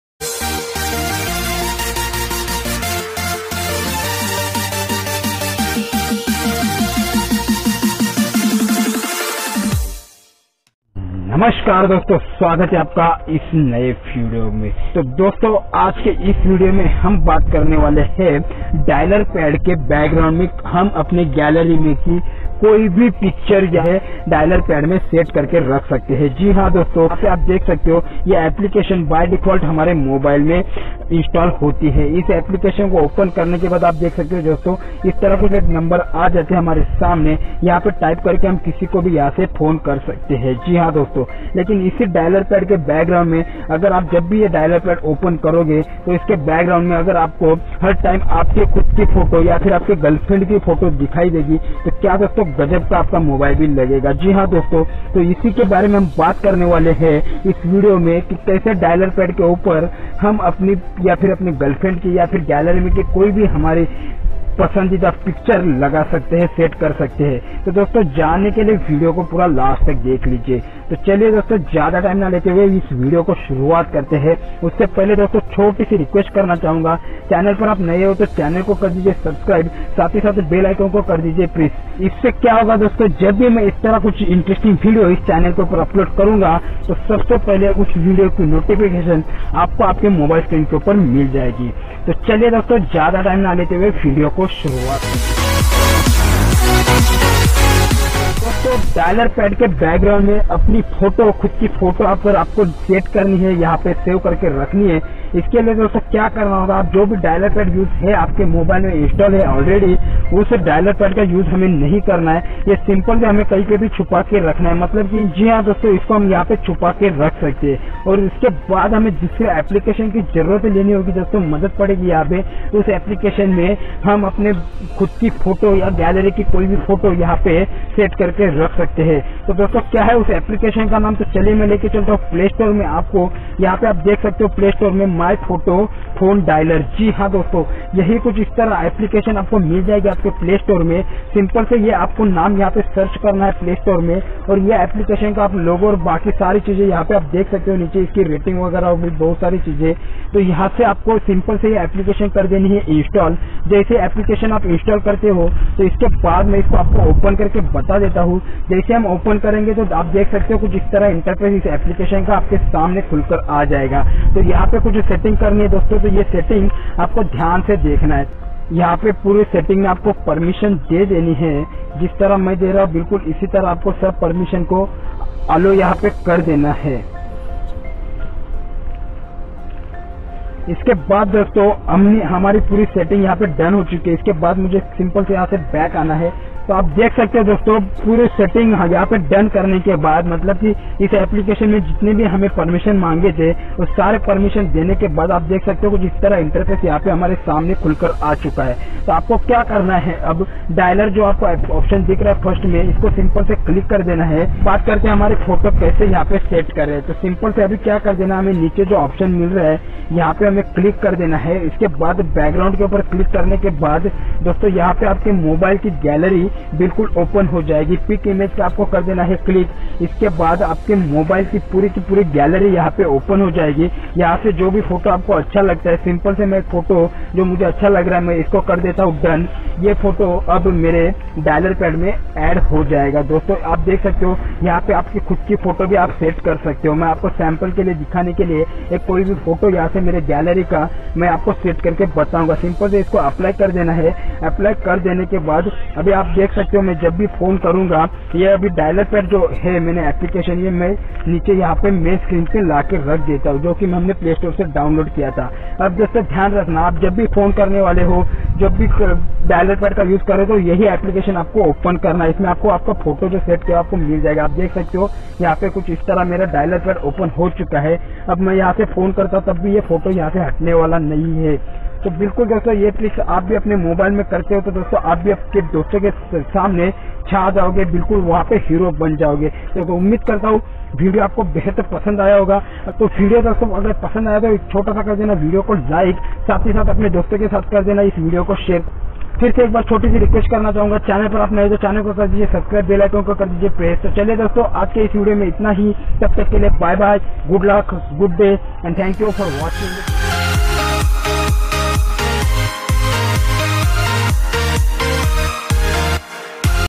Phone Dailer Me lagaye Apani Khud Ki Photo.phone dialler secret trick.
Namakar Dosto.

Is video me maine aapko bataya hai kaise hum apane phone ke Dailer pad par Apani Khud ki photo set kar sakate hai.Ummed karta hu dosto Video aapako behad pasand ayega.

DHANYAWAD.